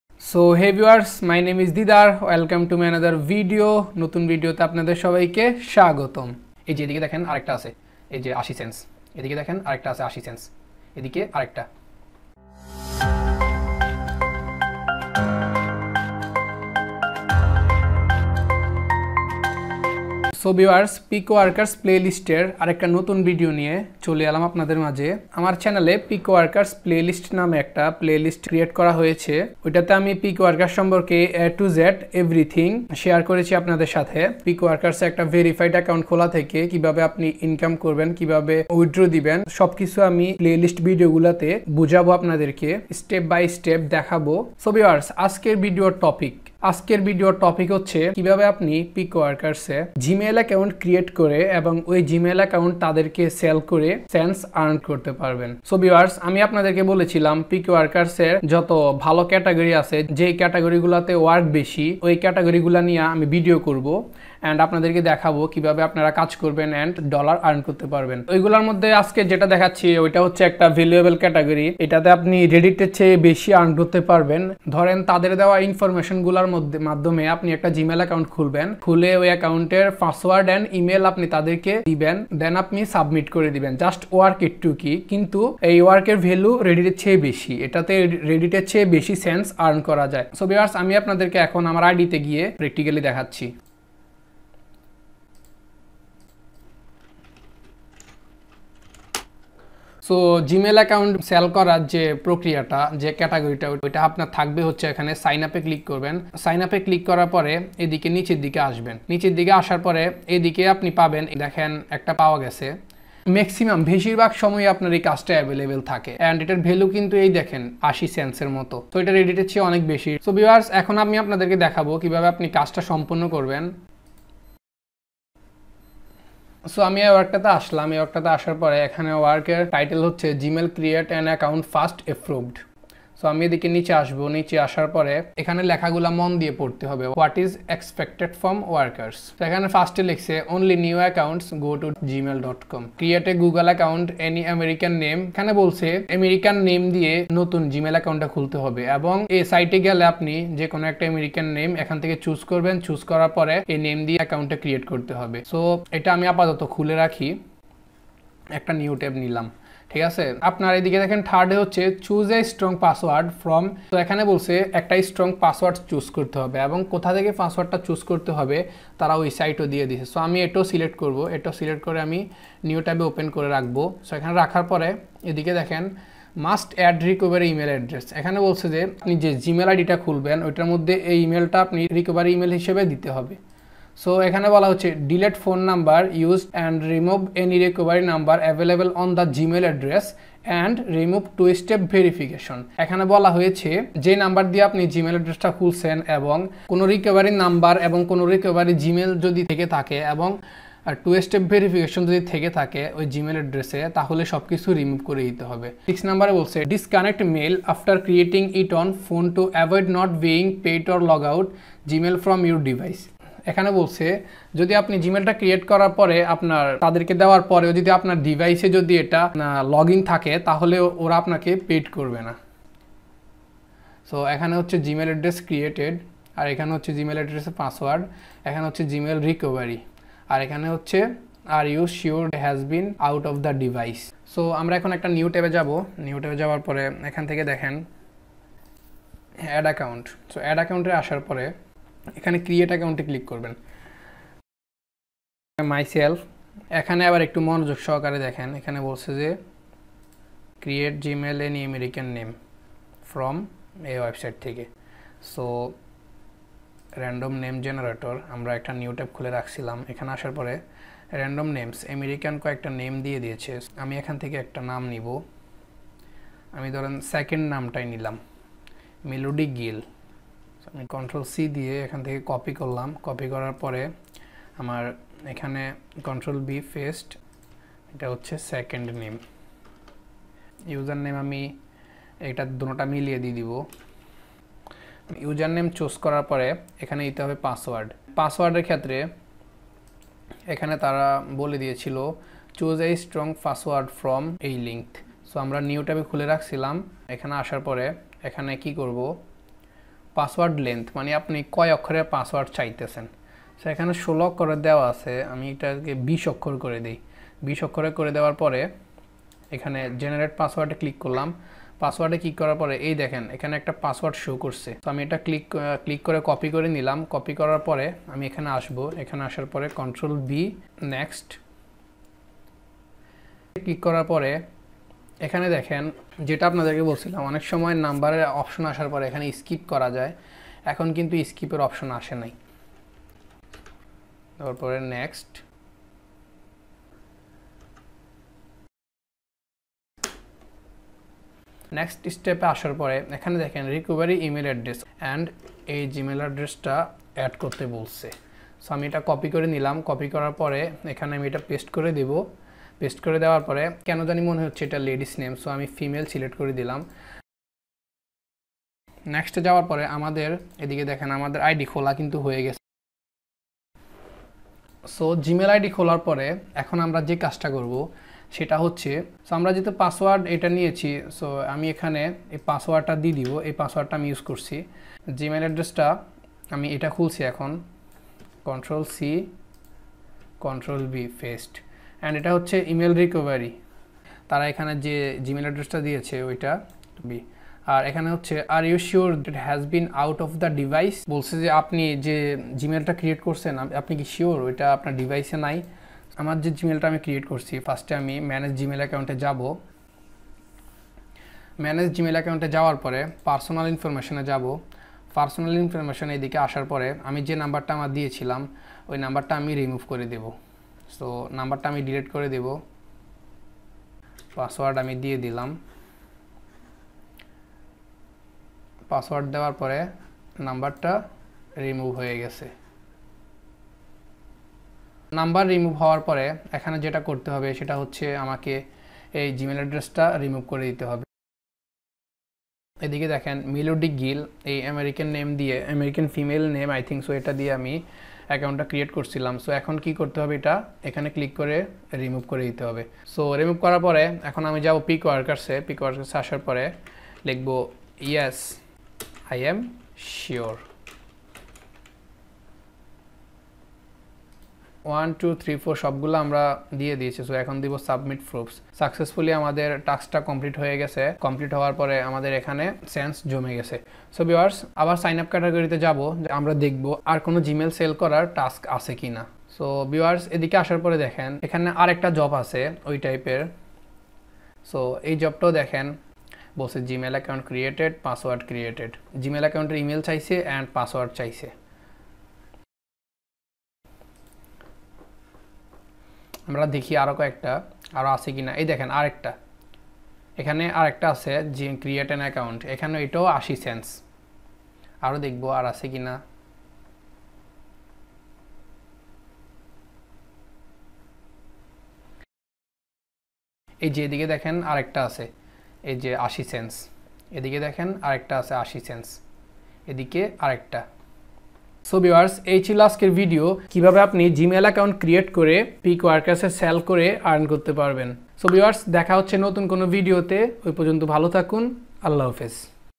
How To Sell Gmail Account In Online Platform | Earn Money To Sell Gmail ID | Picoworkers
How to sell unlimited Gmail address in the Microworkers site Picoworkers. create Gmail account with client given information and submit all information's what client wants, and do the work successfully and earn your money,

Picoworkers e Kivabe clients er deoya steps follow kore gmail id khole sell korben and gmail id er somosto information's client k dia tasks submit korben ei video te sob dekhano hoyeche, video ti valo lagle like comnt and subscribe kore pashe thakon poroborti sokol video dekhar jonno.